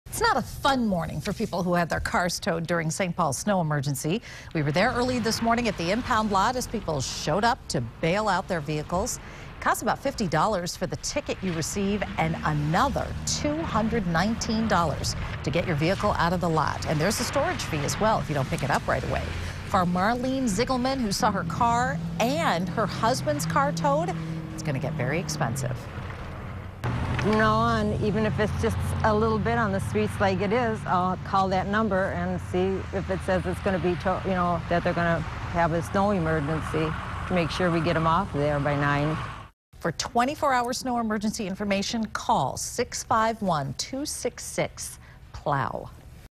VIDEO: Snow Emergency Towing
It's not a fun morning for people who had their cars towed suring St. Paul's snow emergency.